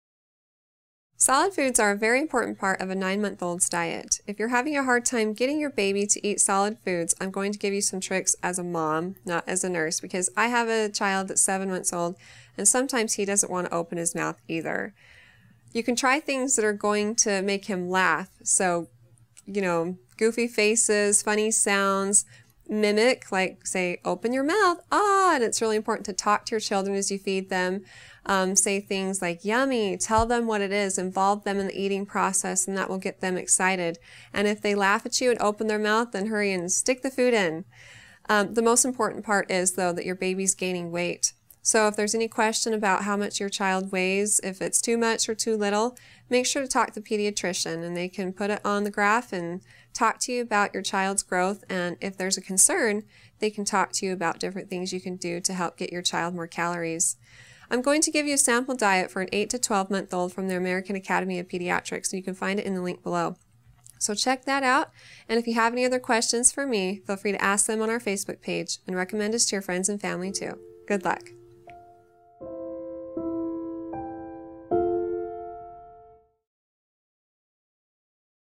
How do I get my baby to eat rice cereal and oatmeal?
Solid foods are a very important part of a 9 month old's diet.  If you're having a hard time getting your baby to eat solid foods I'm going to give you some tricks as a mom, not as a nurse, because I have a child that is seven months old and sometimes he doesn't want to open his mouth either.  You can try things that are going to make him laugh, so goofy faces, funny sounds, mimic, like say, open your mouth, ahh.  It's really important to talk to your children as you feed them.  Say things like yummy.  Tell them what it is, involve them in the eating process and that will get them excited.  If they laugh at you and open their mouth then hurry and stick the food in.  The most important part is though that your baby is gaining weight.  If there is any question about how much child weighs, if it's too much or too little, make sure to talk to the pediatrician and they can put it on the graph and talk to you about your child's growth and if there is a concern they can talk to you about different things you can do to help get your child more calories.  I'm going to give you a sample diet for an 8-12 month old from the American Academy of Pediatrics.  So check that out and if you have any other questions for me feel free to ask them on our Facebook page and recommend us to your friends and family too.  Good luck.